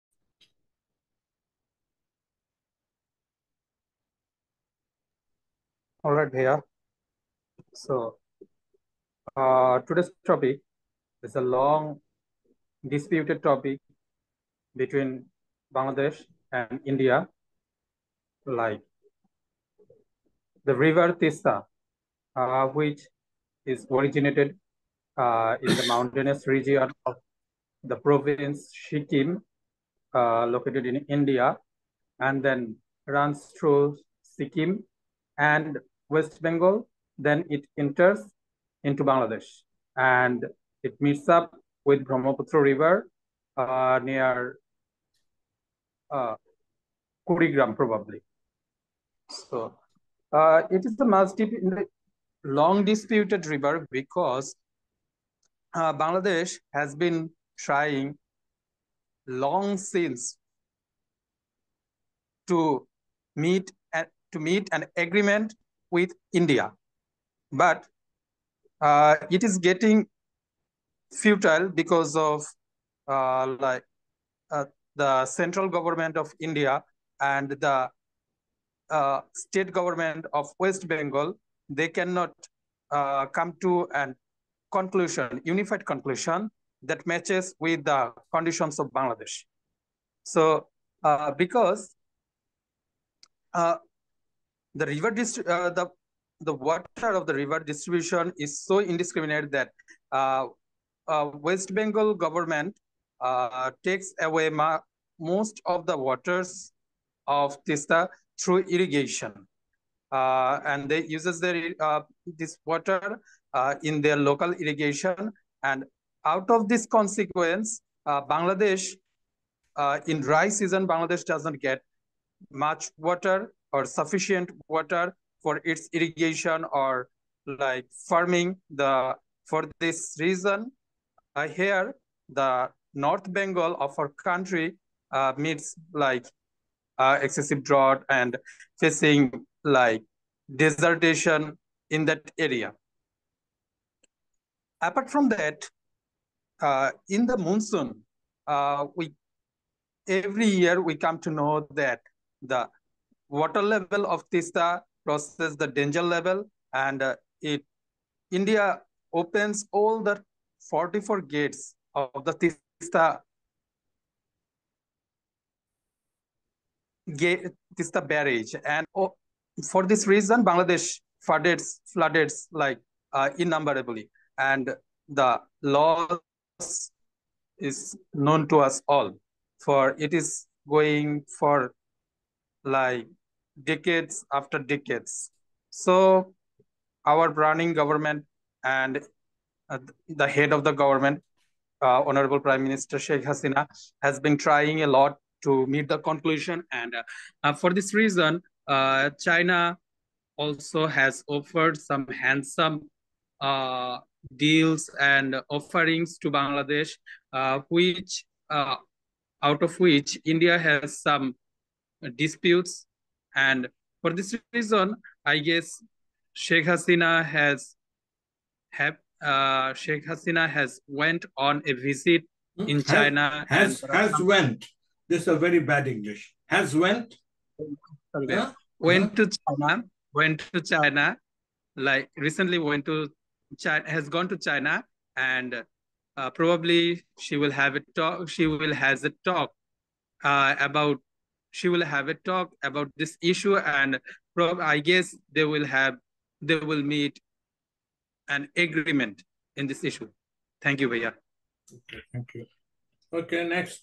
Adda in English Bangladesh #9 July 2024
Imran Haider talking about the Teesta River